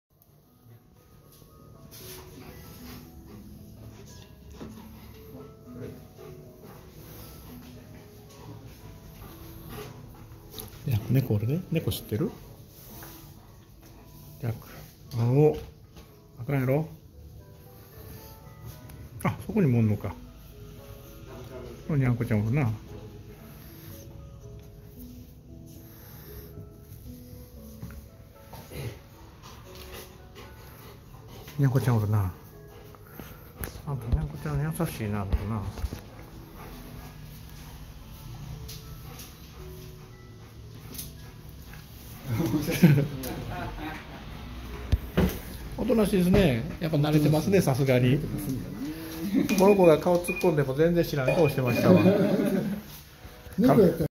『いらっしゃいませ、きょうはどうしましたか？』ジャック君病院で猫と対面。
ジャック君を獣医さんに連れて行った時の様子です。
病院で暮らす猫が２匹。
犬に対しては穏やかですが、猫には怒ったりするそうです。
ジャック君が猫と一緒に遊んだことがあるかは聞きそびれましたが、襲うような素振りは有りません。
受付嬢の猫ちゃんに気を取られてたら、左側にももう一匹会計担当の子が居ました。